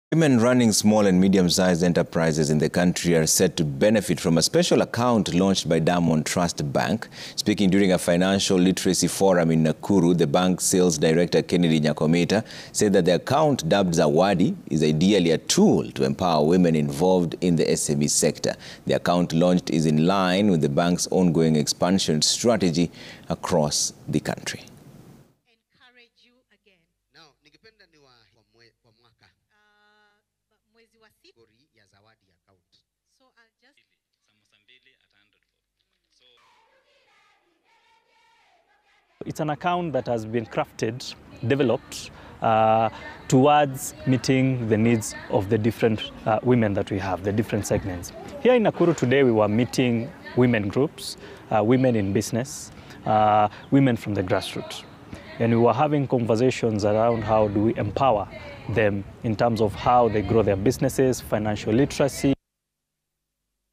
Diamond Trust Bank launches Zawadi for women running SMEs
Women running small and medium sized enterprises in the country are set to benefit from a special account launched by Diamond Trust Bank.
Speaking during a financial literacy forum in Nakuru, the bank's sales director Kennedy Nyakomitta, said the account dubbed Zawadi is ideally a tool to empower women involved in the SME sector. The account launch is in line with the bank's ongoing expansion strategy across the country.